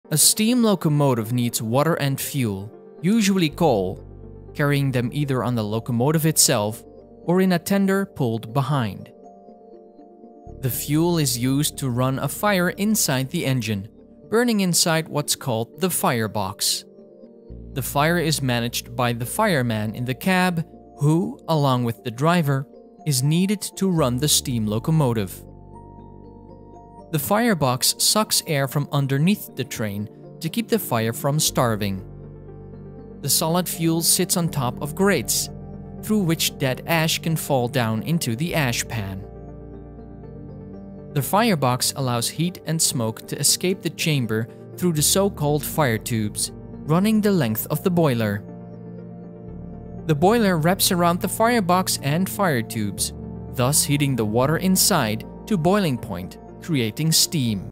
How does a train🚆 work
The Video show cases on how a steam engine train work, and this is a 1st part of many parts to come.